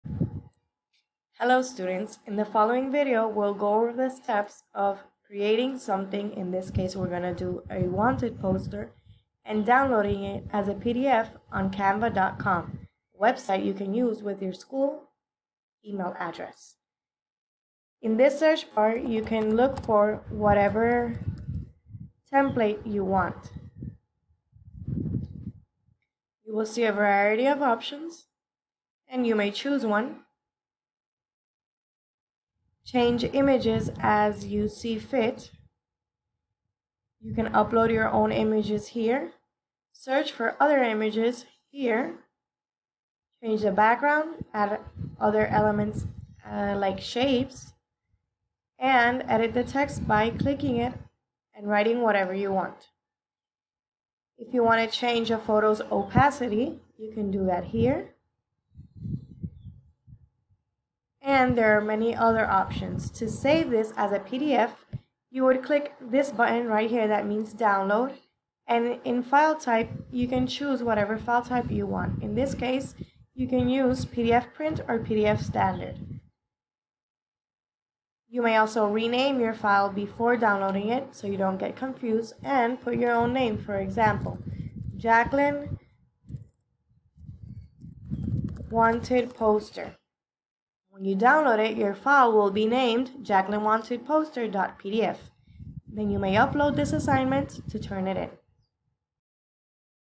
Create a Poster or Other Item on Canva.com and Select Save Format (PDF, PNG, GIF, etc)
This video will show you how to search for templates, edit your design, and save in the format asked for (we will use PDF here).